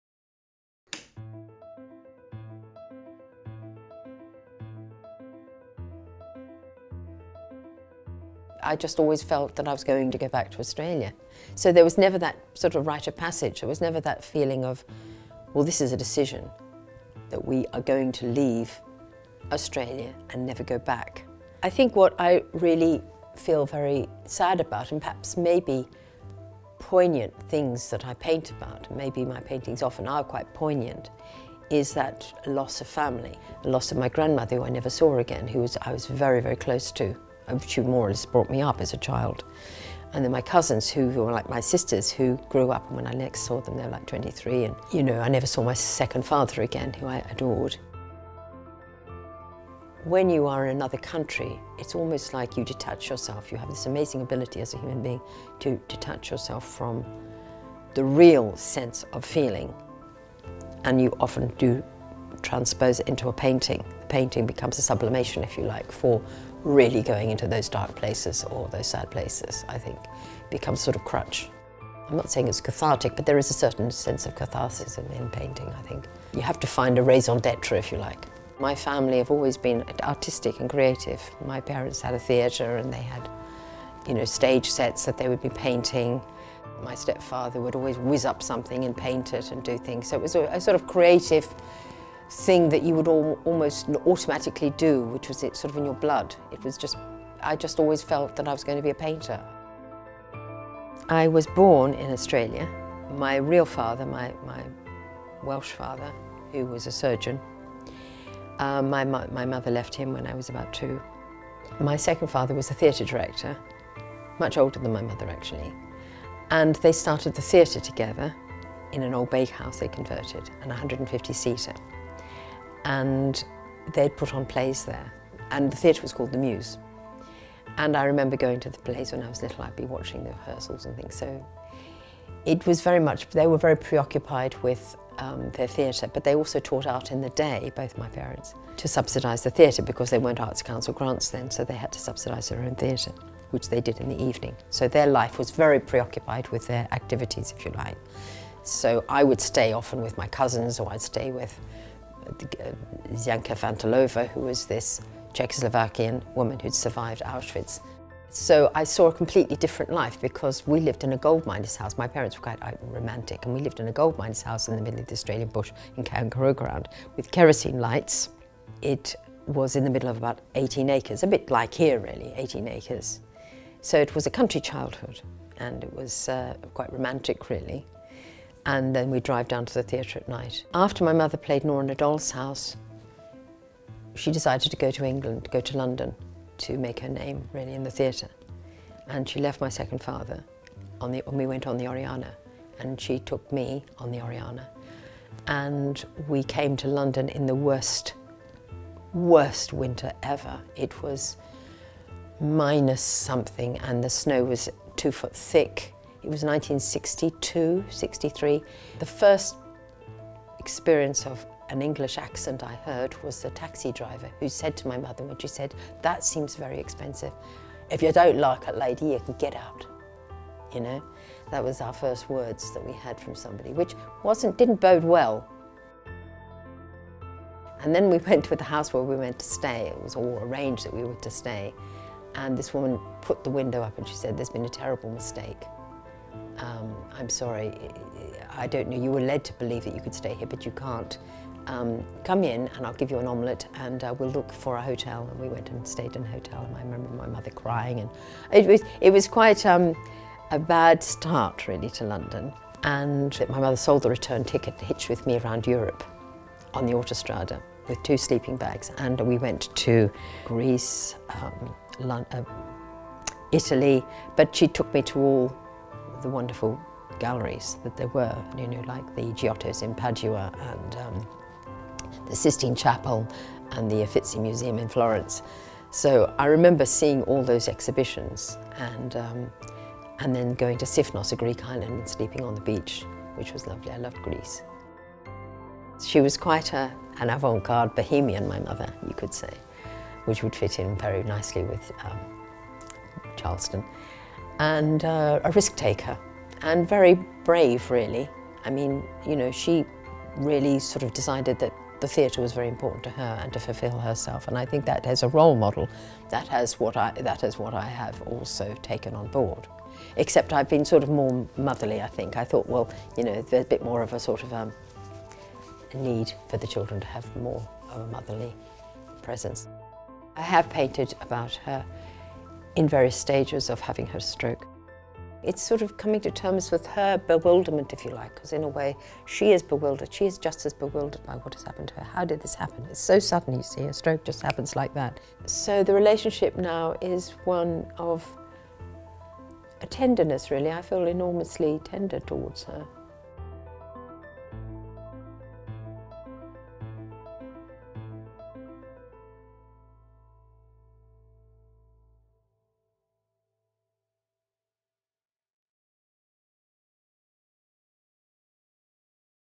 Shani Rhys James on Family | Artist Interview
From her early childhood in Australia to growing up in London with her actor mother, acclaimed artist Shani Rhys James discusses how she paints and reveals some of the memories, relationships, experiences and emotions that influence her work.

Shani Rhys James: tea on the sofa, blood on the carpet (1 February - 19 April 2020) is a major exhibition of the artist's latest body of work in which she explores the transience of being; contrasting her early iconic painting of a child's cot with startling images of the female figure ravaged by decay.